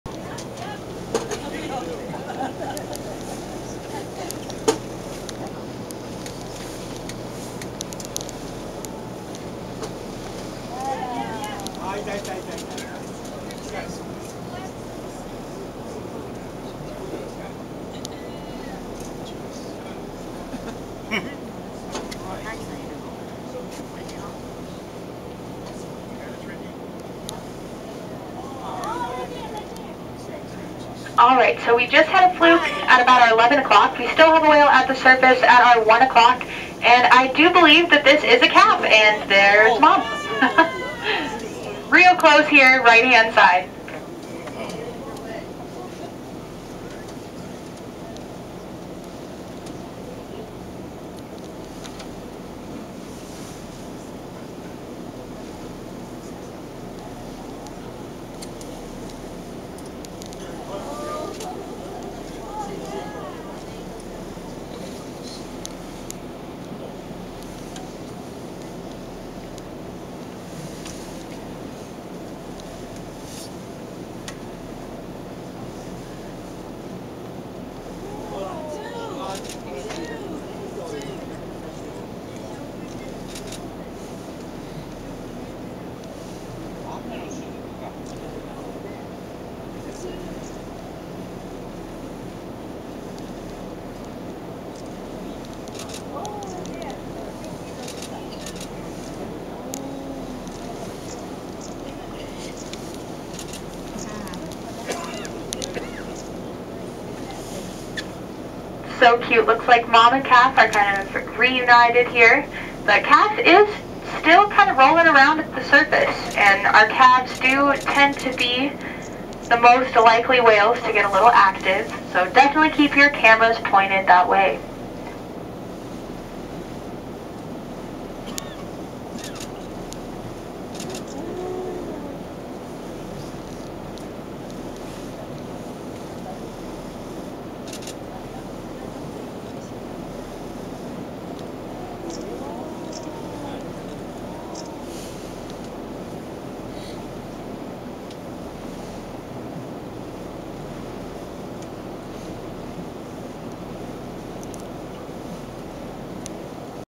【クジラ】アラスカ・シトカから小型船での海洋生物を見に行く船に乗り換えた。 ロイヤル・プリンセス号で大黒埠頭⇒桜前線を追いかけ宮古から北海道⇒太平洋横断⇒アラスカ全２４日間クルーズツアー。
クジラ：USAアラスカ・シトカから小型船での海洋生物を見に行く船に乗り換えた。
　ロイヤル・プリンセス号で大黒埠頭⇒桜前線を追いかけ宮古から北海道
　　　　　⇒太平洋横断
　　　　　　⇒アラスカ氷河クルーズ全２４日間。